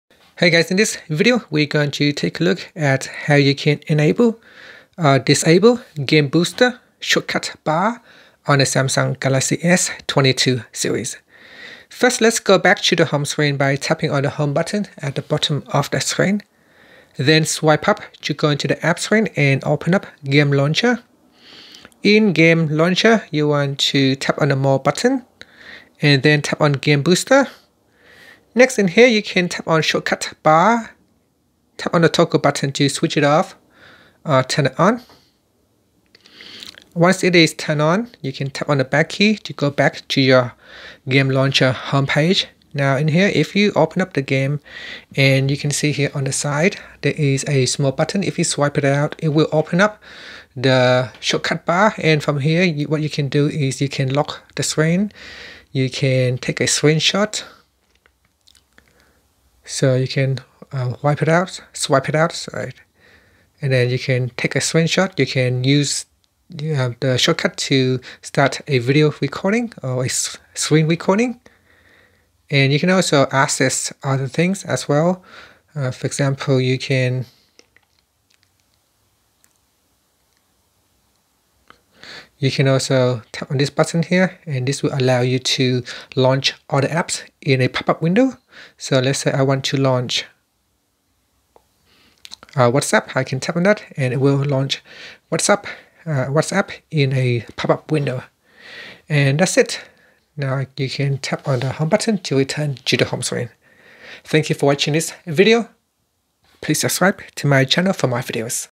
Galaxy S22/S22+/Ultra: How to Enable/Disable Game Booster Shortcut Bar
Learn how you can enable or disable game booster shortcut bar on the Samsung Galaxy S22 / S22+ / S22 Ultra.